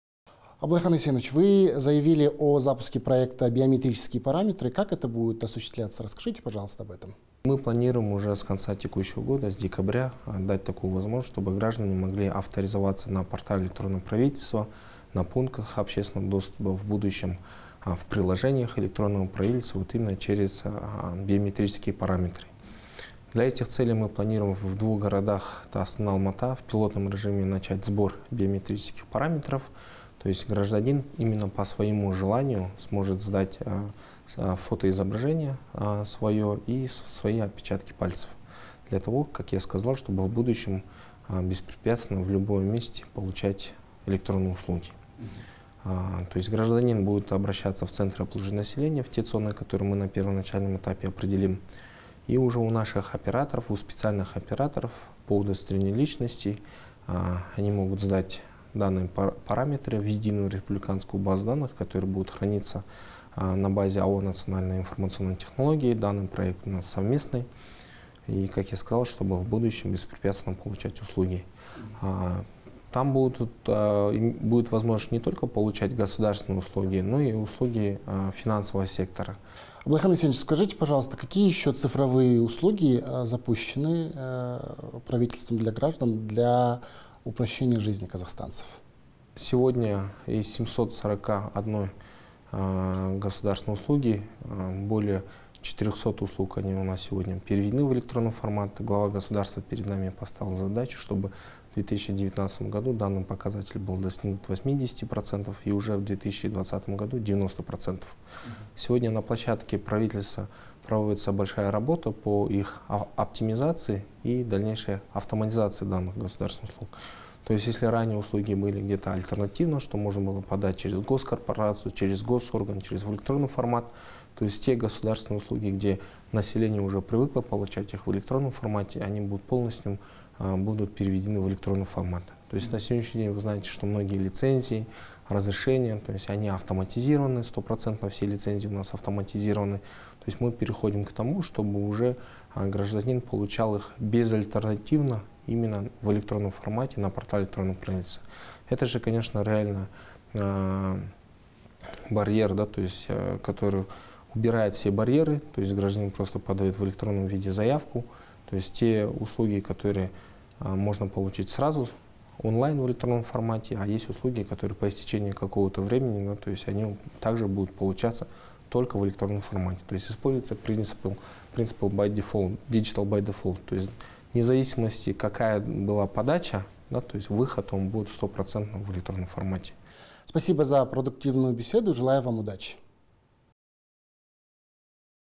Как будет проводиться сбор отпечатков пальцев казахстанцев (22.11.18)
Собирать биометрические данные граждан начнут уже в декабре 2018 года. Процедура добровольная, говорят эксперты. В пилотном режиме проект реализуют в Алматы и Астане. Подробнее об этой кампании телеканалу «Алматы» рассказал глава госкорпорации «Правительство для граждан» Аблайхан Оспанов